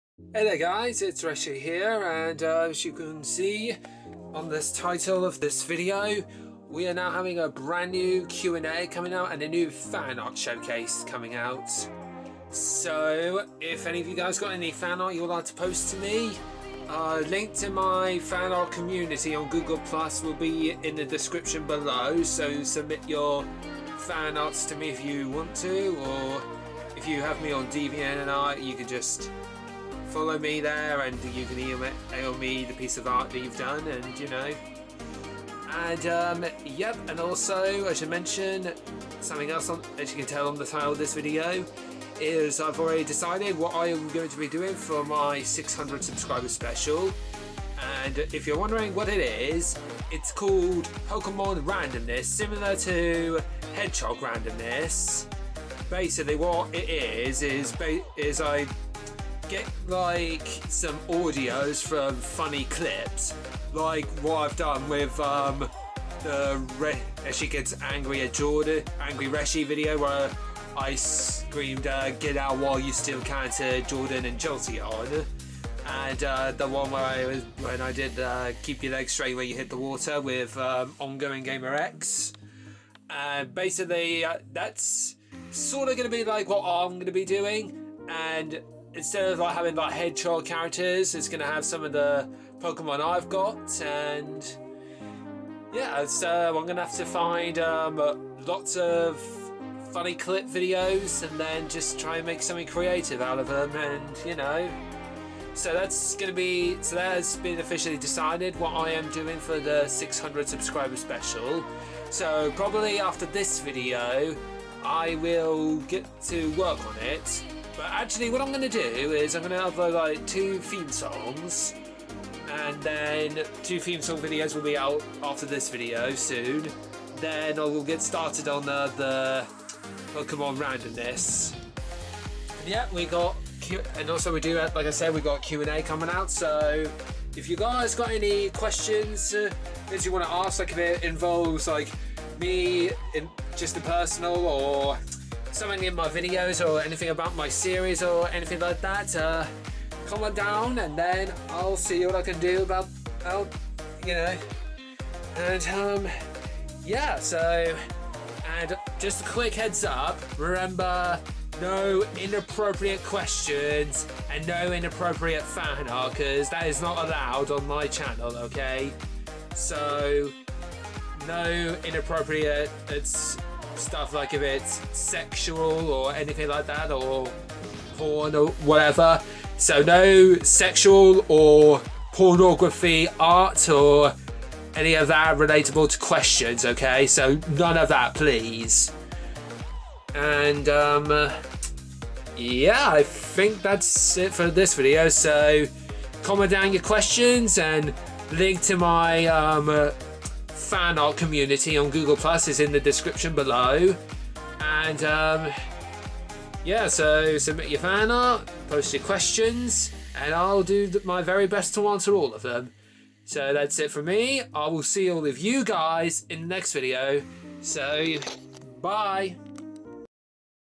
New Q and A and fan art showcase announcment (and 600 subscriber special decided!)
Ask anything and make any art you want but please no inappropriate questions or fan art thank you